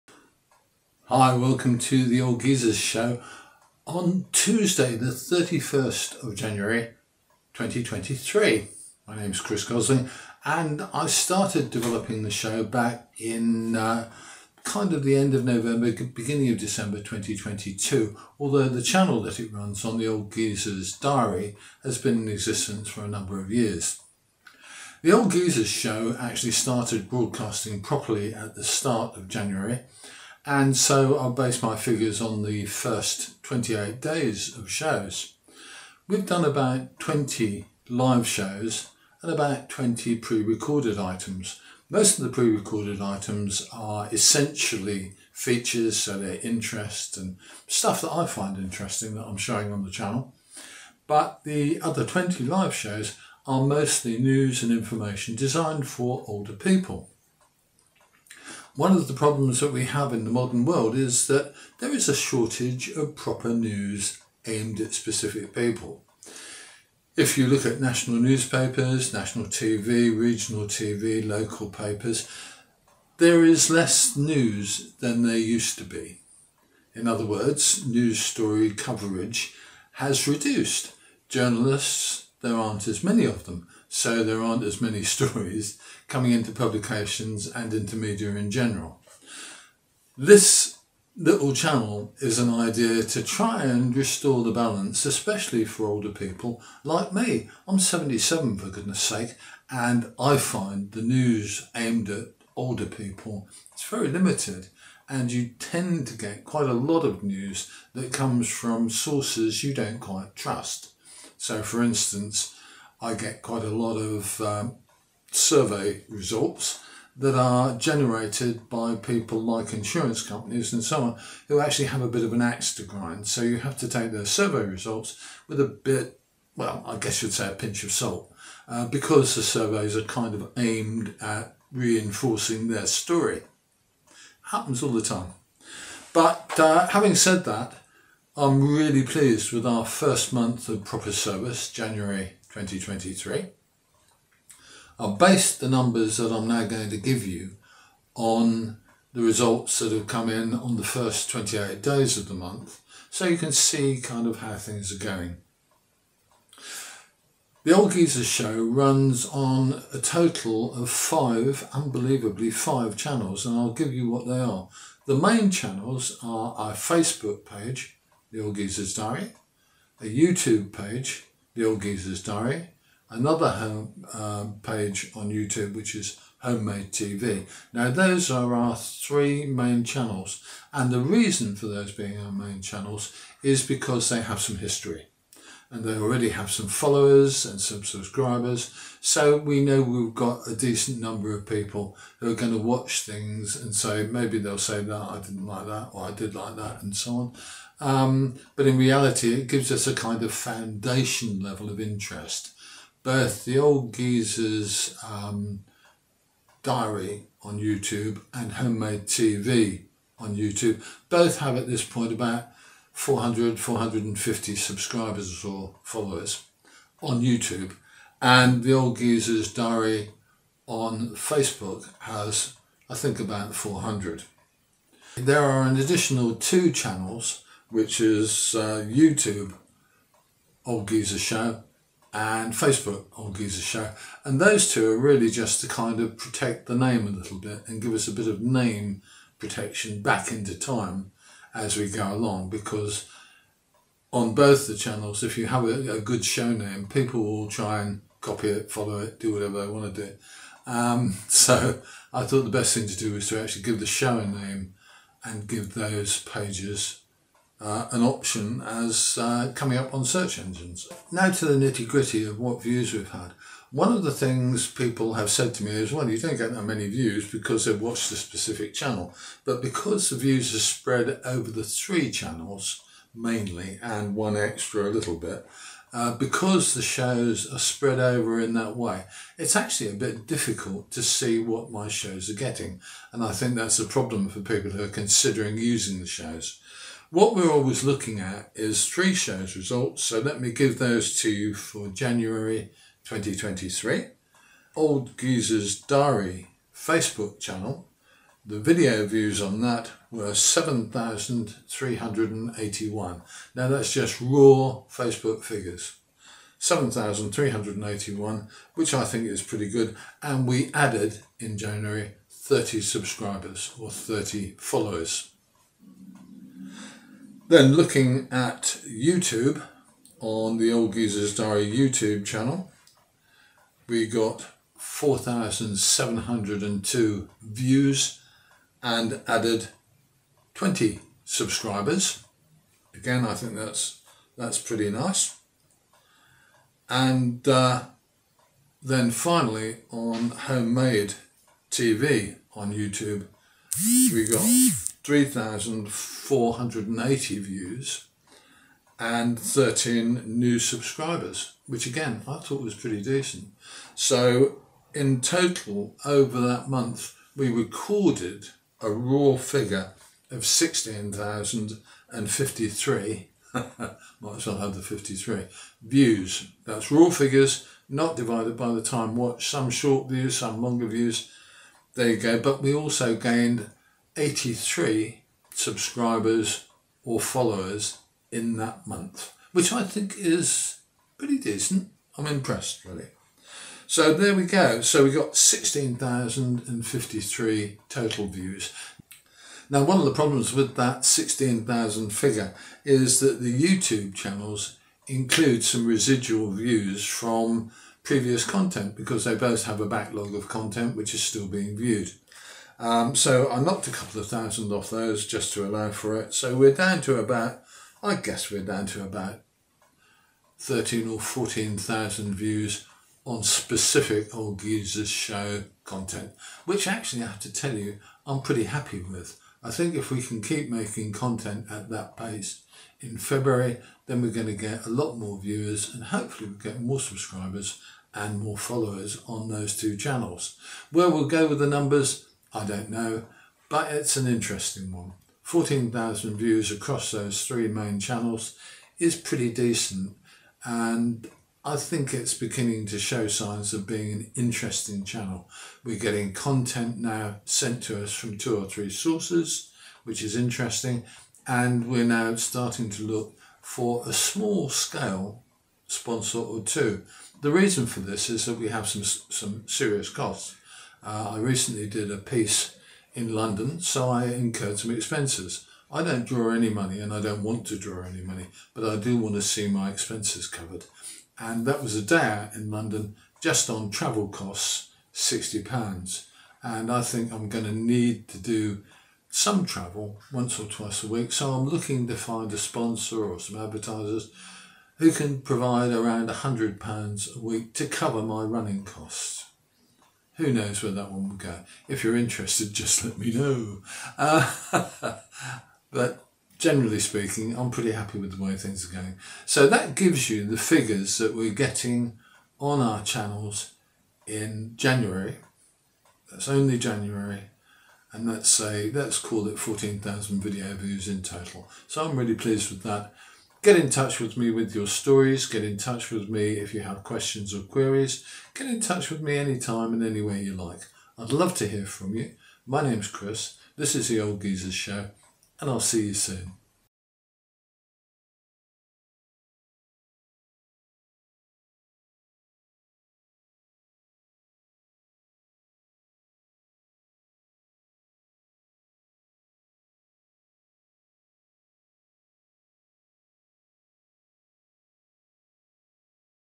January 2023 - first month's views report from the Old Geezer Show
WATCH - January 2023 - first month's views report from the Old Geezer Show
The Old Geezer Show has just completed its first month of proper service, with a set of viewing and engagement figures that really impress me . . . .
So we'll be off with another month's worth of production starting tomorrow!